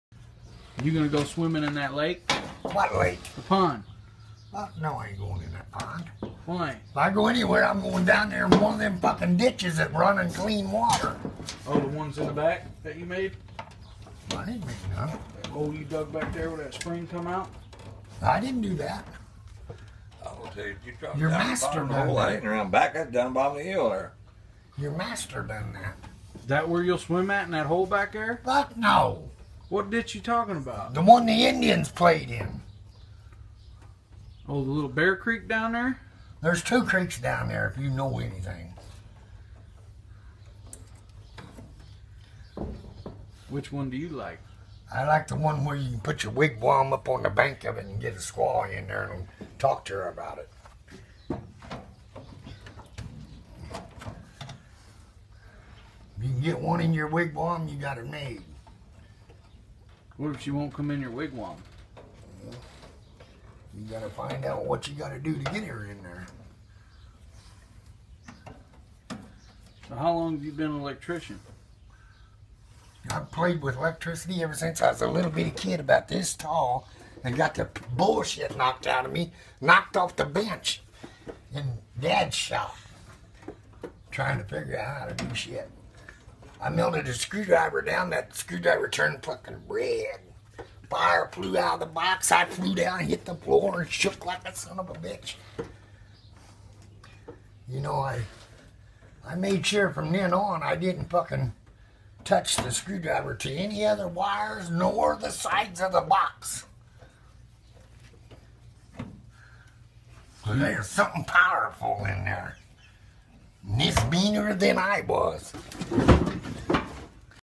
ac wiring
Chet tells of his first encounter with electricity, and what he learned not to do. He also gives the viewer insight on women.